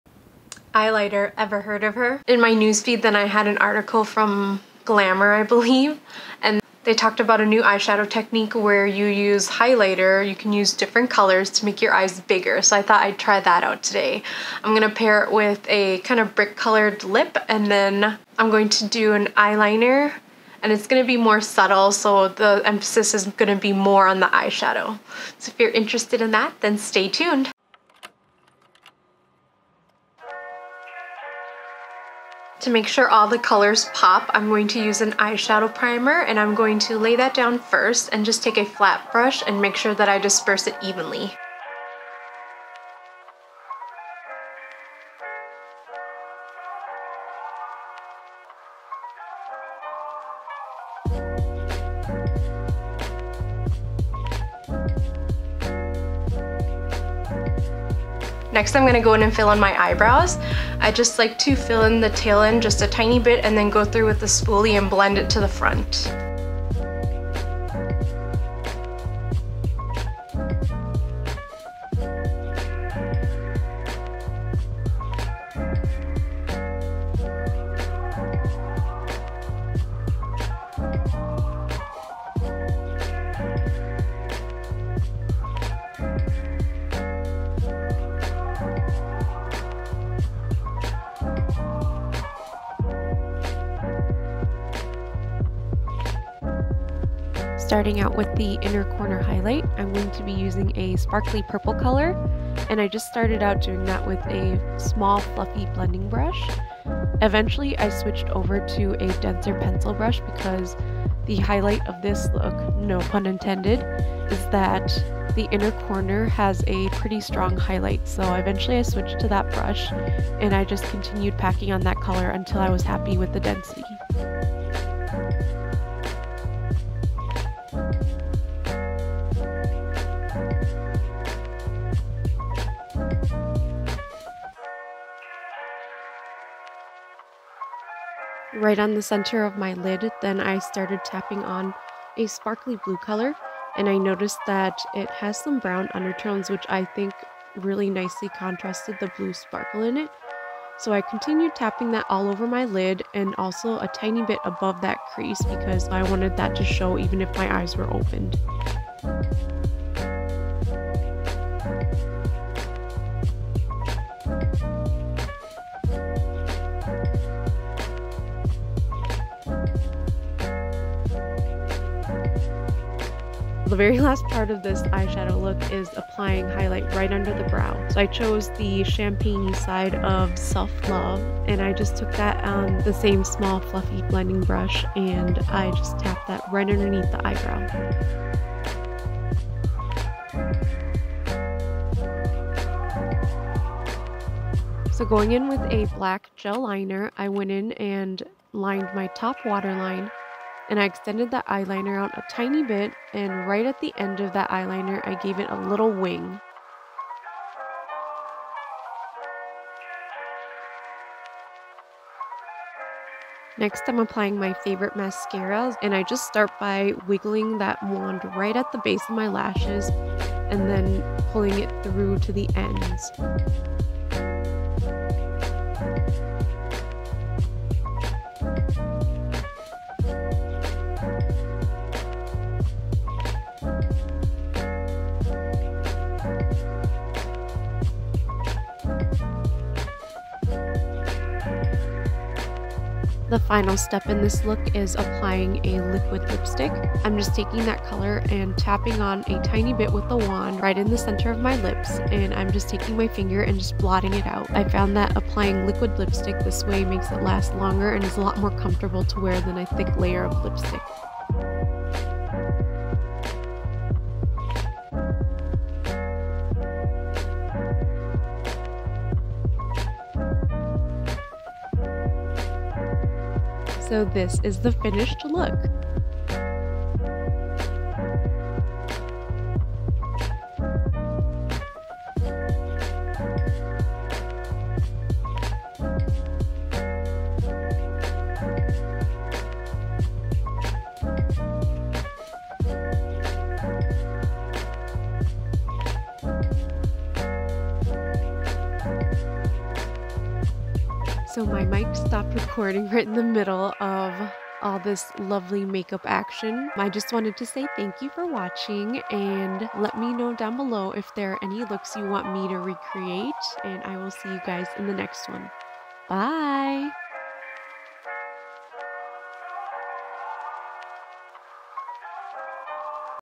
Eyelighter the new trend that will make your eyes bigger| A guide to the simple makeup trick
Eyelight, the new trend that is meant to make eyes look bigger with the strategic placement of highlighter on certain parts of the eye. Watch as I guide you in this tutorial the easy steps to get this easy and glowy look with minimal effort and maximum impact. Iridescent shimmers help highlight and shape the eyes with the help of gel liner to round and define eyes. I finished the lip with a brick color that I blotted for everyday wear. I kept the face natural because my skin has been sensitive so feel free to dress up your face however you’d like!

Hi! I am a regular person just like you hoping to have some fun with beauty. So come along with me as we try to figure how to makeup!

Products used:
Eyeshadow primer L.A. Girl HD Pro Primer Eyeshadow Stick in White
Brow pencil Loreal Brow Stylist in Brunette
Brushes Sigma Brushes and BhCosmetics
Palette Huda Rose Quartz Palette
Japonesque Power curler
Mascara Maybelline rocket volum" in black waterproof
Eyeliner Colourpop Crème Gel Liner in Swerve
Lip Maybelline SuperStay Matte Ink in Cocoa Connoisseur

Get 1 month free with the link